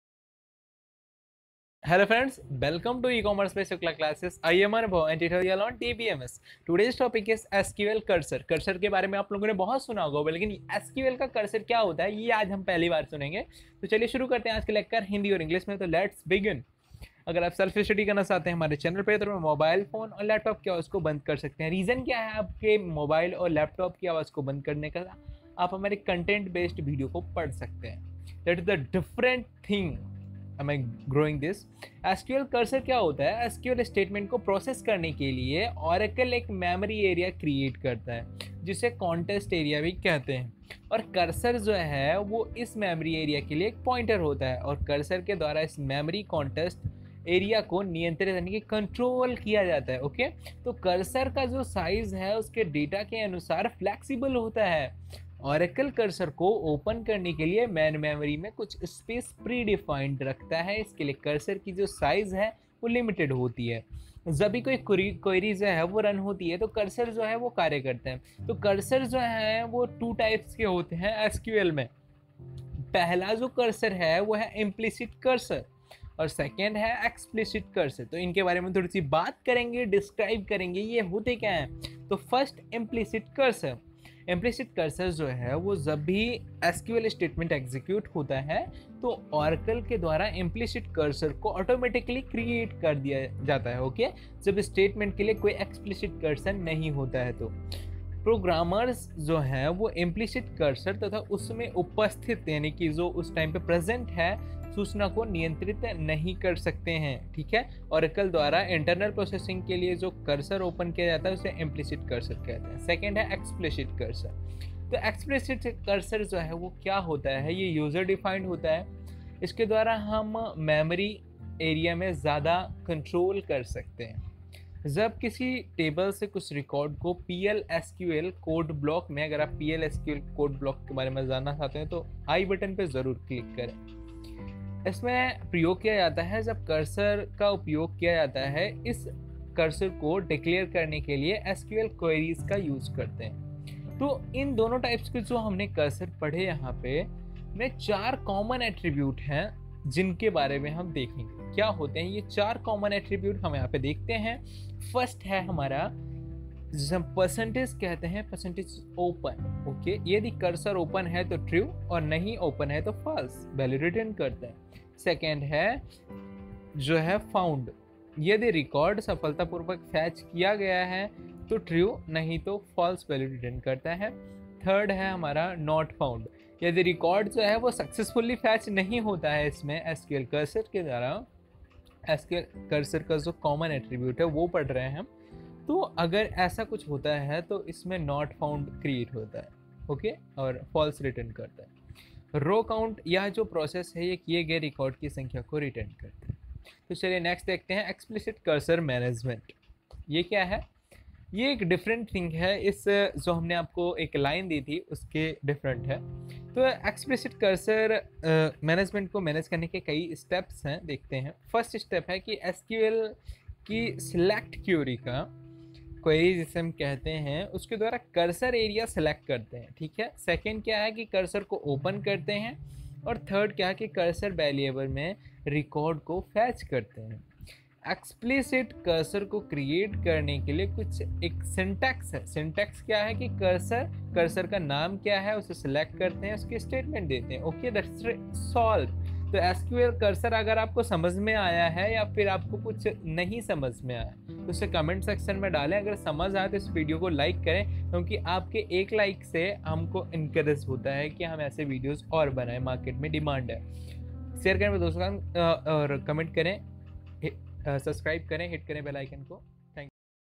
SQL Cursor || Hindi and English || SQL Cursor tutorial || SQL Cursor in Hindi
in this video about SQL cursor complete step by step problem solve of SQL cursor.